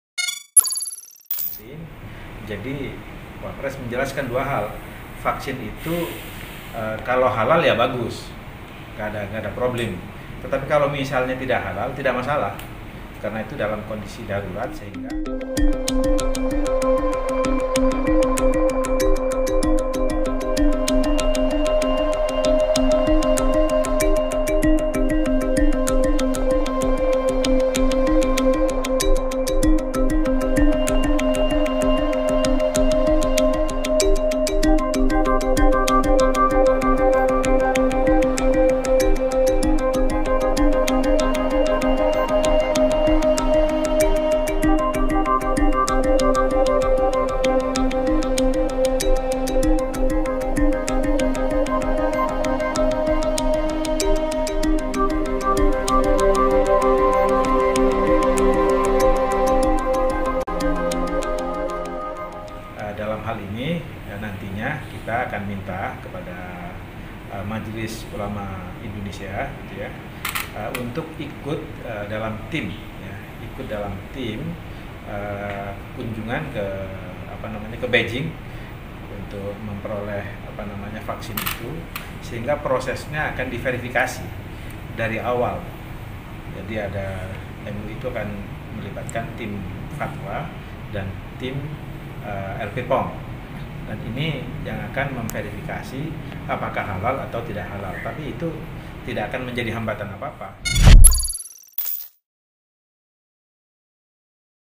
Jubir Wapres: Vaksin Tak Halal Tak Masalah Karena Darurat!
Wakil Presiden Ma'ruf Amin menekankan poin penting mengenai vaksin COVID-19 dan status kehalalannya. Dalam pandangan Ma'ruf, vaksin COVID-19 tak jadi masalah jika tidak halal karena dalam kondisi darurat.
"Oleh karena itu, tadi juga dilaporkan oleh Pak Luhut ya bahwa vaksin ini bagaimana soal kehalalannya kepada Wapres. Maka Wapres menjelaskan hal yang penting jadi Wapres menjelaskan dua hal. Vaksin itu kalau halal ya bagus nggak ada problem, tetapi kalau misalnya tidak halal tidak masalah, karena itu dalam kondisi darurat sehingga kemudian apa namanya tidak masalah dipakai juga," kata juru bicara Wakil Presiden Ma'ruf Amin, Masduki Baidlowi, dalam keterangannya, Jumat (2/10/2020).

Masduki menyampaikan hal tersebut usai pertemuan Ma'ruf Amin dengan Wakil Ketua Komite Penanganan COVID-19 dan Pemulihan Ekonomi Nasional Luhut Binsar Pandjaitan hingga Menteri Kesehatan Terawan Agus Putranto. Dalam pertemuan itu, Luhut melaporkan perkembangan vaksin COVID-19.

"Yang pertama bahwa vaksin itu akan datang tak lama lagi sekitar bulan November mereka akan berangkat ke China untuk melakukan pengecekan terhadap vaksin yang akan dikirim ke Indonesia, pada tahap pertama kira-kira itu akan datang sebanyak 3 juta vaksin tapi 3 juta ini diperuntukkan bagi tenaga-tenaga kesehatan, TNI-Polri, dan orang-orang yang selama ini berhadapan-bertugas untuk menghadapi masyarakat semuanya terkait penanganan pandemi ini, baru berikutnya akan datang secara bergelombang sangat banyak," ujar Masduki.

Selain itu, kata Ma'ruf, pemerintah akan meminta MUI berkunjung ke Beijing terkait vaksin COVID-19 ini. Kunjungan itu untuk memverifikasi perihal proses pembuatan vaksin di China.

"Dalam hal ini, kita akan minta MUI untuk ikut dalam tim kunjungan ke Beijing untuk memperoleh vaksin itu, sehingga prosesnya akan diverifikasi dari awal, jadi ada melibatkan tim fatwa dan tim LPPOM, ini yang akan memverifikasi apakah halal atau tidak, tapi itu tak akan menjadi hambatan apa-apa, karena kalau halal alhamdulillah prosesnya begitu saja, tapi kalau nggak halal pun nggak masalah karena darurat sehingga diperbolehkan jadi ini tak akan menghambat apapun. Itulah yang dijelaskan oleh wapres," ujar Ma'ruf.

"Maka jangan ada anggapan kemudian proses kehalalan vaksin itu akan menghambat, itu sama sekali tidak menghambat karena sekali lagi saya tegaskan kalah tidak halal itu juga darurat, nggak masalah," sambung dia.